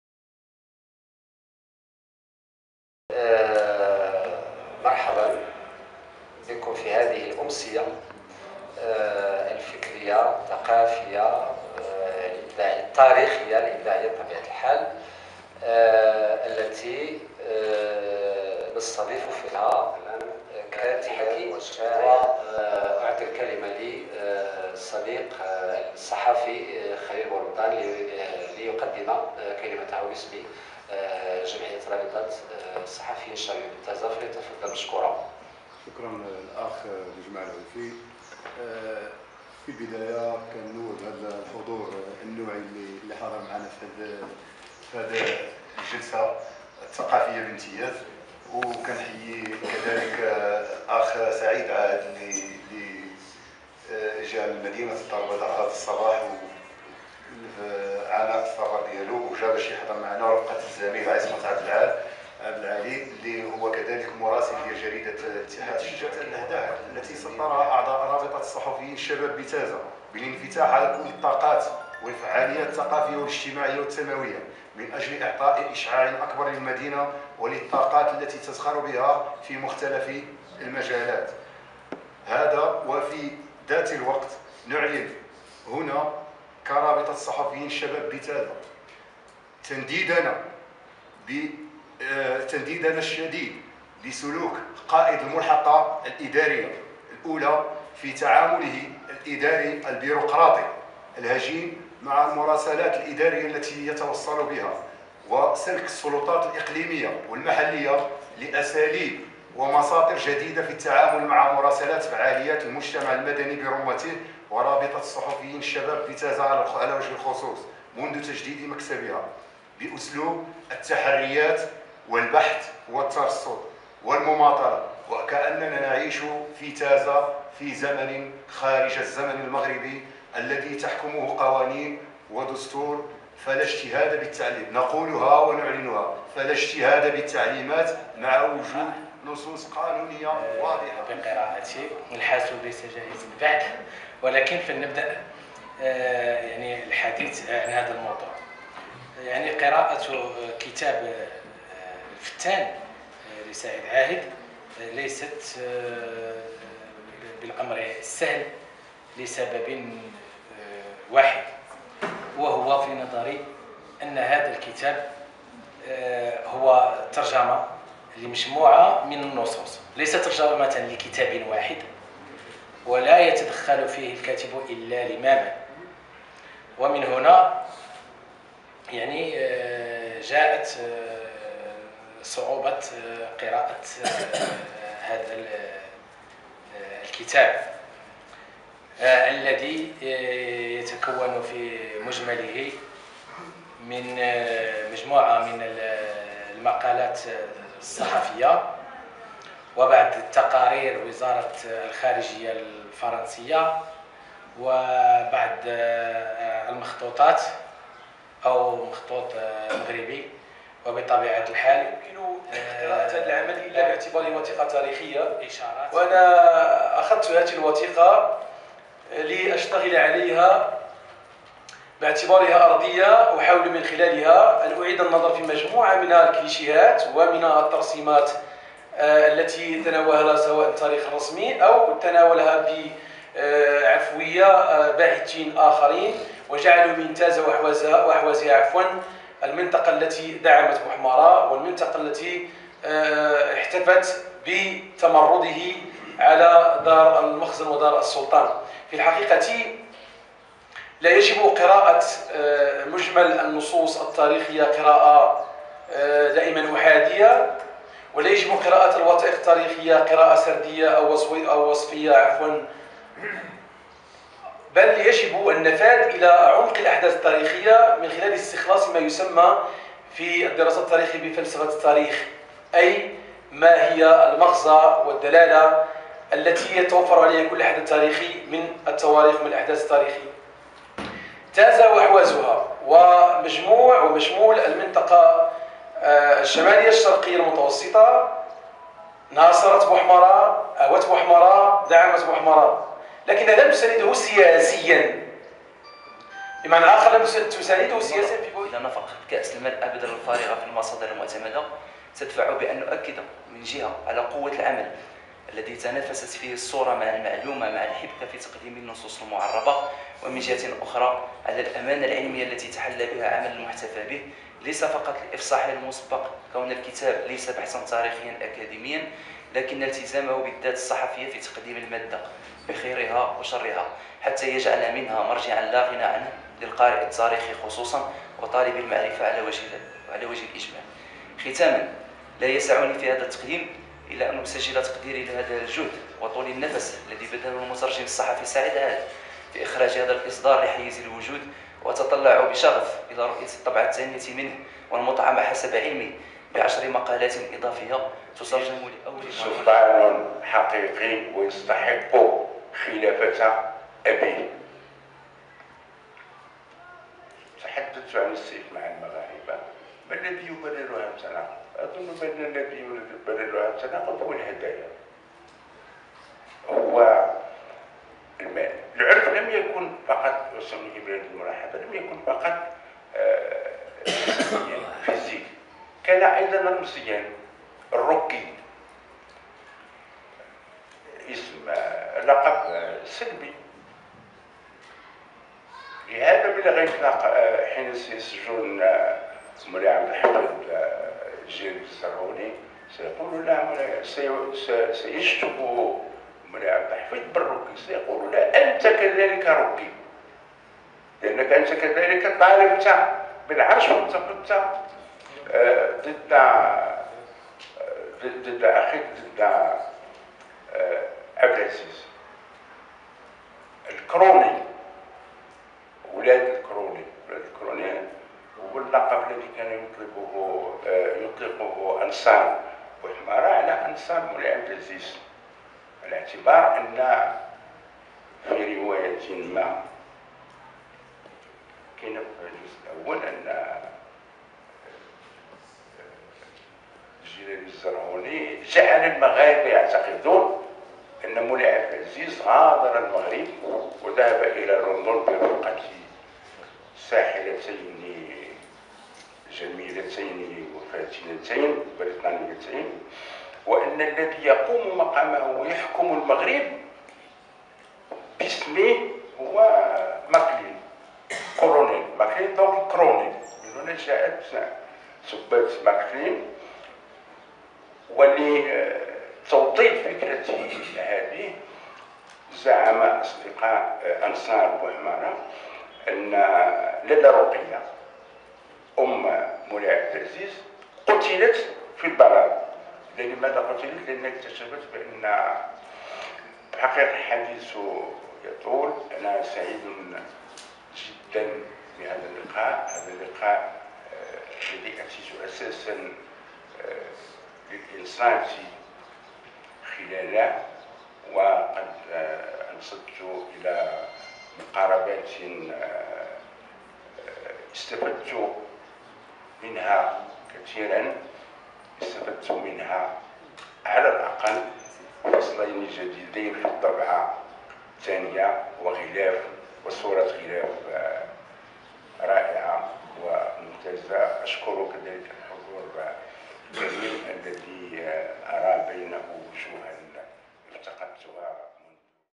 من حفل تقديم إصدار 'الروكَي بوحمارة' بتازة
ضمن أنشطتها الفكرية و الثقافية، تستضيف رابطة الصحفيين الشباب - تازة بدعم من جهة تازة الحسيمة تاونات و بتعاون مع الجماعة الحضرية لتازة و الفرع الجهوي لاتحاد كتاب المغرب تازة و جمعية أصدقاء تازة، الصحفي و المترجم "سعيد عاهد" في حفل تقديم و توقيع آخر إصداراته "الفتّان: محكيات من سيرة الروگي بوحمارة لصحفيين وكتاب غربيين معاصرين له" بمشاركة كل من د.محمد العزوزي ــ د.بوجمعة العوفي ــ د.عياد أبلال ــ ذ.عادل فهمي وذلك يوم السبت 03 ماي 2014  (ابتداء من الساعة السادسة مساء) بقاعة الاجتماعات التابعة للجماعة الحضرية بتازة.